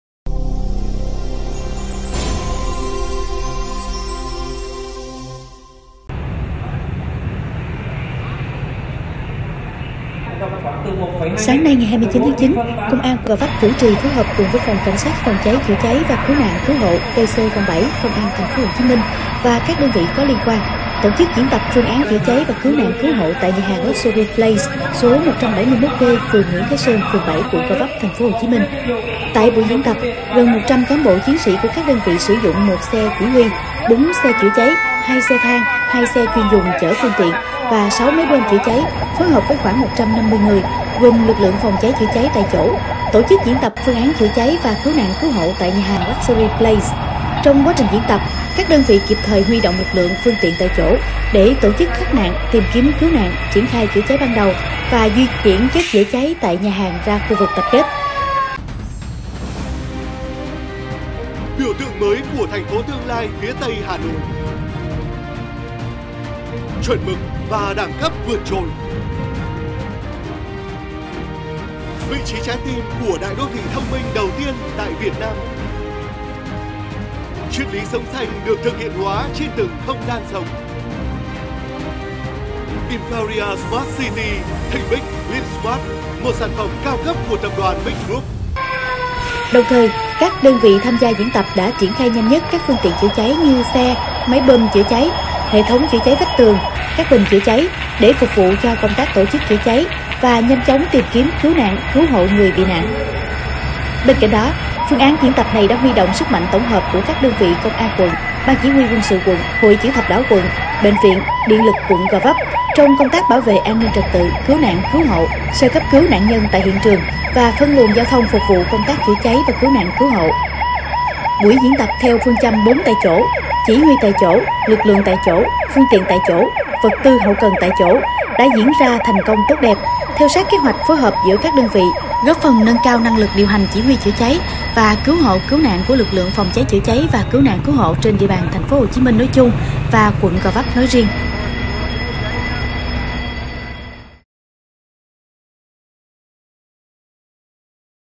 HCM: Diễn tập PCCC tại quận Gò Vấp
Tầm nhìn TV: Sáng 29-9, Công an quận Gò Vấp chủ trì, phối hợp với Phòng Cảnh sát Phòng cháy, chữa cháy và cứu nạn, cứu hộ (PC07) - Công an TP Hồ Chí Minh và các đơn vị có liên quan, tổ chức diễn tập phương án chữa cháy và cứu nạn, cứu hộ tại Nhà hàng Luxury Place, số 171D đường Nguyễn Thái Sơn, phường 7, quận Gò Vấp, TP HCM.